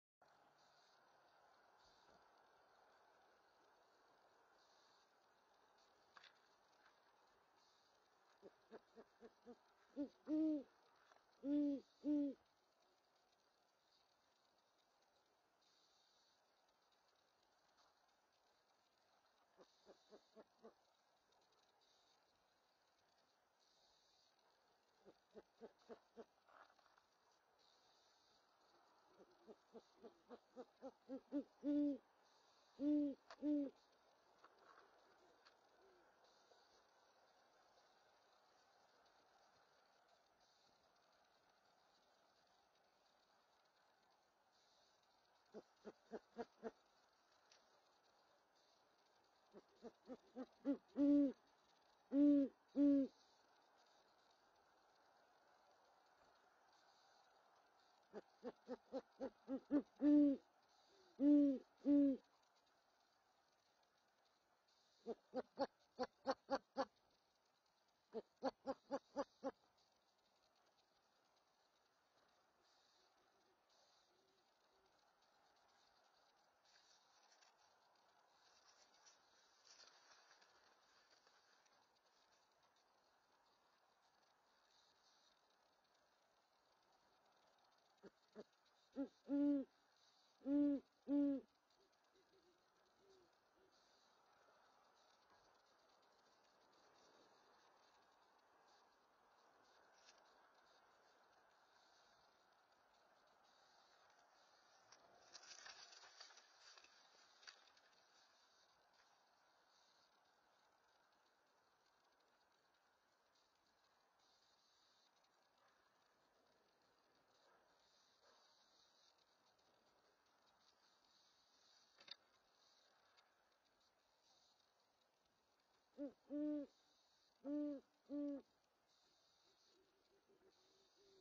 Great Horned Owl!
The very first visit to our new Nest Cam!!